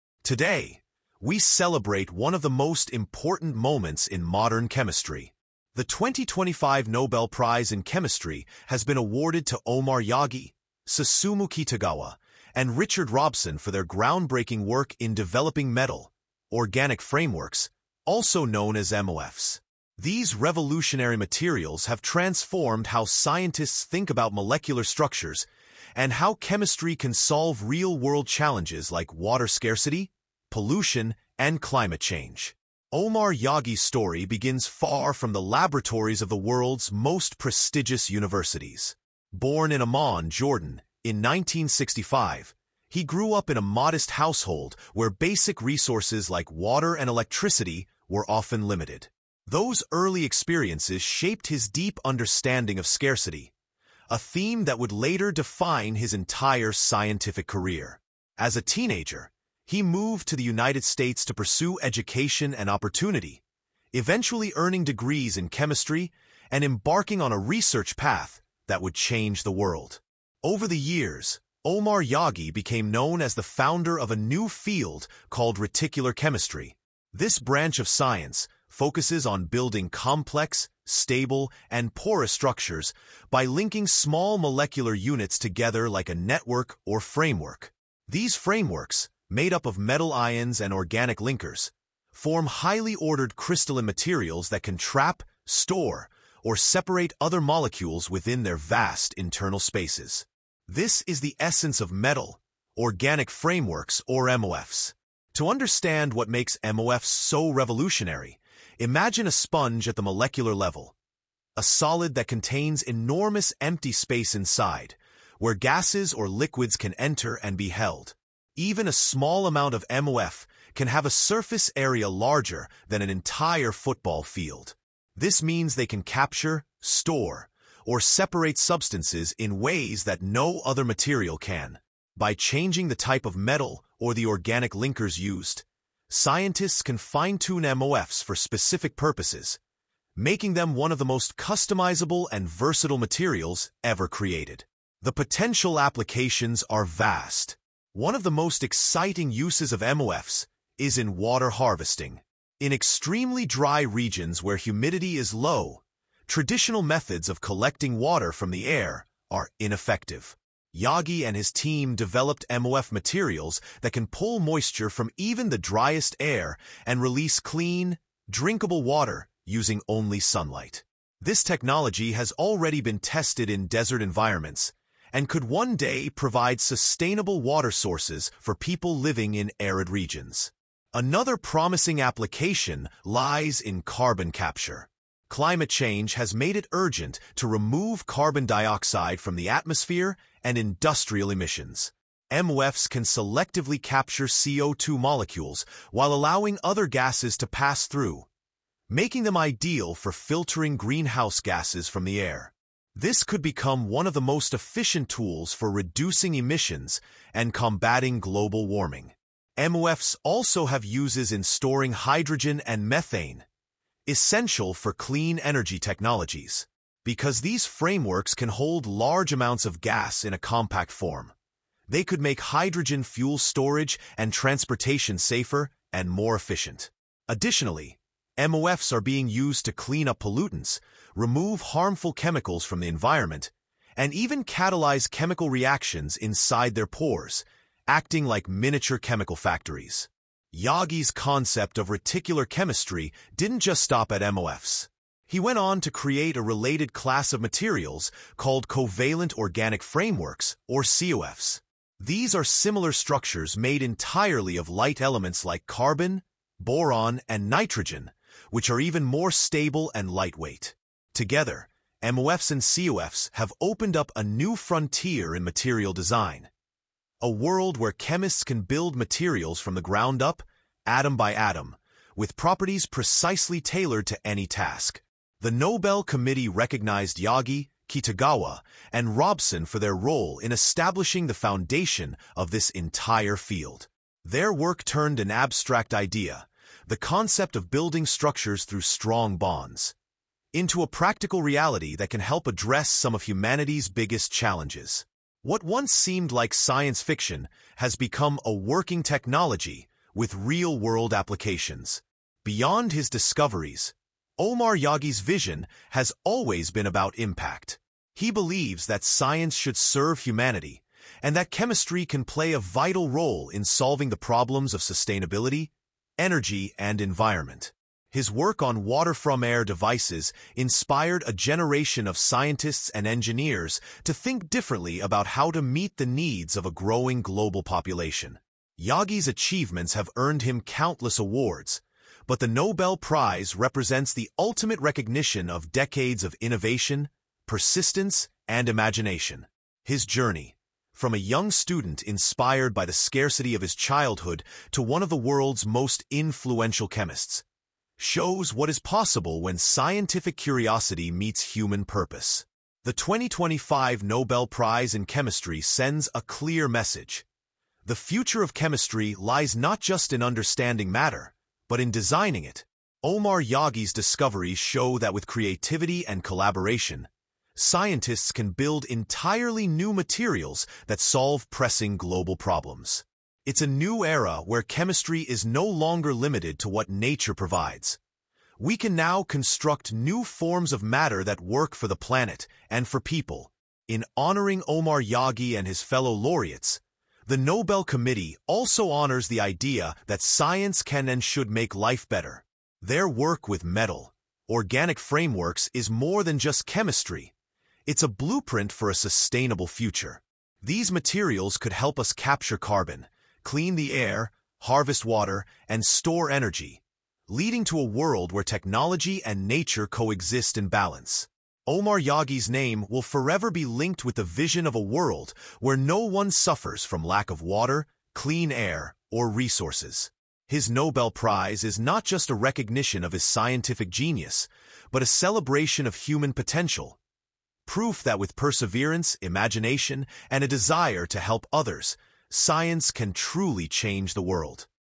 Omar Yaghi Wins 2025 Nobel Prize in Chemistry | The Genius Behind Metal–Organic Frameworks
The 2025 Nobel Prize in Chemistry has been awarded to Omar Yaghi, along with Susumu Kitagawa and Richard Robson, for their groundbreaking discovery of metal–organic frameworks (MOFs) — a revolutionary class of materials that could change the future of science, energy, and the environment.

In this video, we explore who Omar Yaghi is, his inspiring journey from humble beginnings in Jordan to becoming a Nobel laureate, and how his work in reticular chemistry has transformed our understanding of molecular design. Learn how MOFs can harvest water from desert air, capture carbon dioxide, and store hydrogen for clean energy.

From solving global water scarcity to tackling climate change, Yaghi’s research is shaping a sustainable future for humanity. Discover how his Nobel-winning science is not just chemistry — it’s a movement toward innovation, resilience, and hope.

✅ Topics Covered:

Omar Yaghi’s life and scientific journey

What are metal–organic frameworks (MOFs)?

How MOFs work and their real-world applications

The science of reticular chemistry

Nobel Prize in Chemistry 2025 significance

Future of sustainable chemistry and global impact